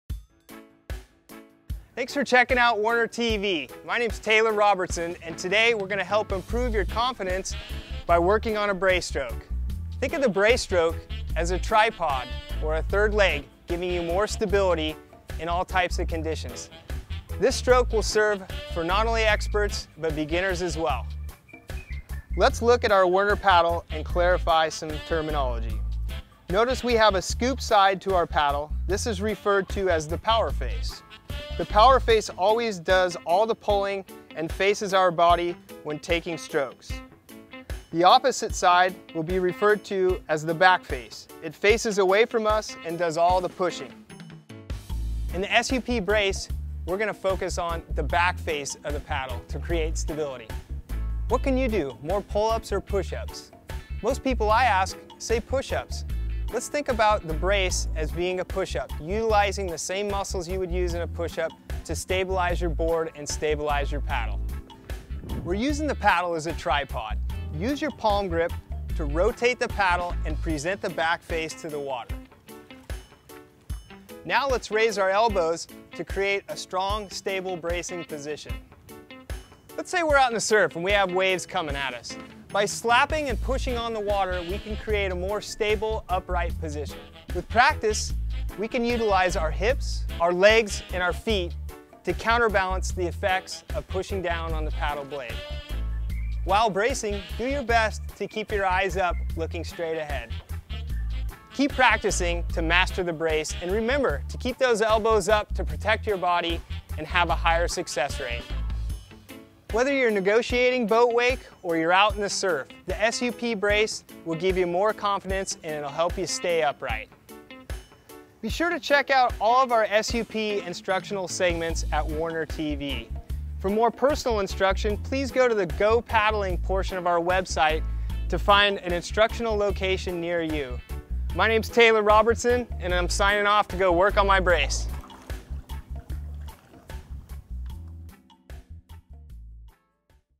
SUP Bracing
Part of our stand up paddle instructional series, this segment focuses on bracing strokes. Taylor takes us through hand and elbow position and how to incorporate your blade and body to stabilize...